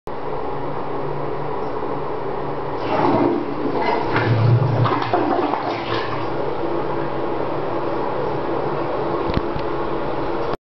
New American Toilets are Flushing Much Faster, Video32-USA2008
Die aktuellen Klobecken spülen echt fix und brauchen viel weniger Wasser.